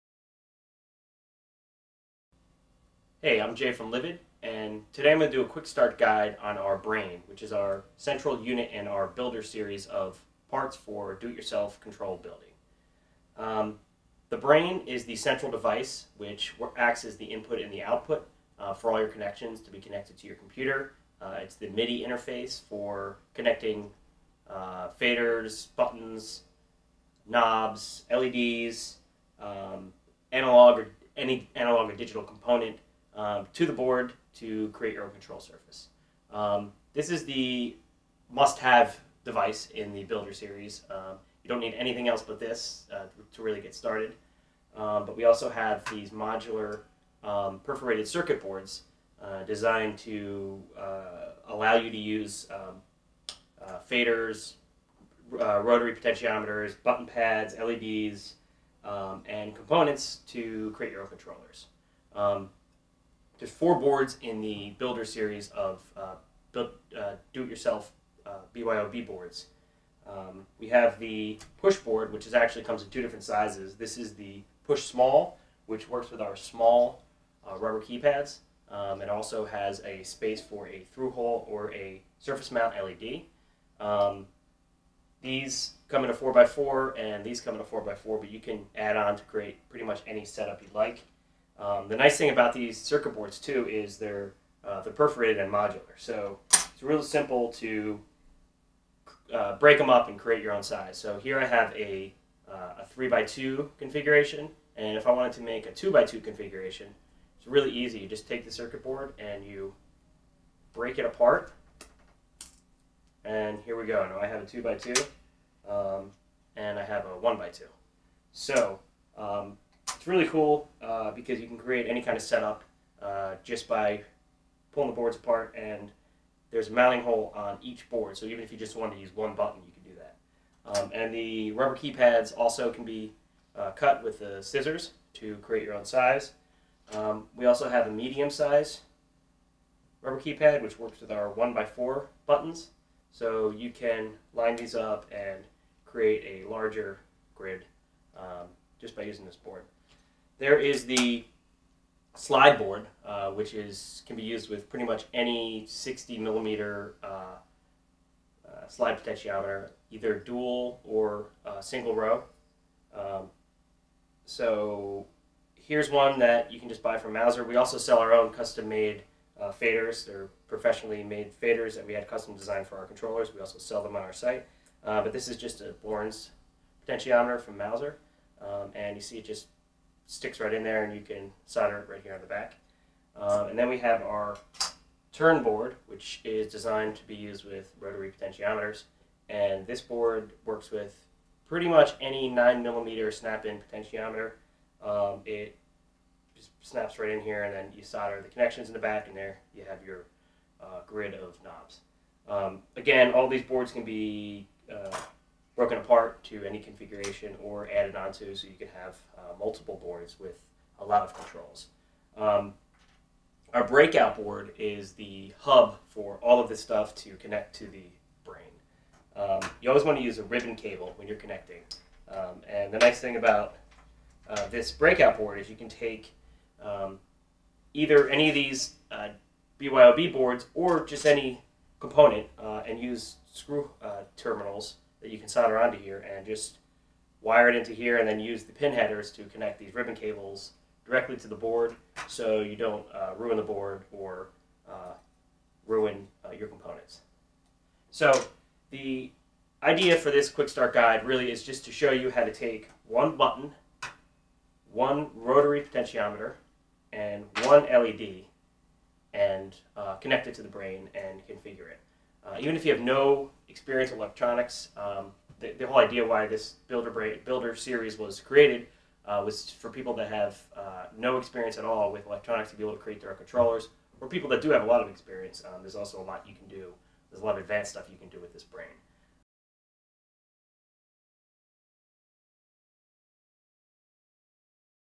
Livid Builder Quick Start Part1
Quick Start guide and overview of the Builder series of DIY modular kits from Livid Instruments.

This is part 1 of a 2 part beginner's guide to the Builder Brain MIDI interface.

Livid Instruments: Handmade in Austin,TX USA.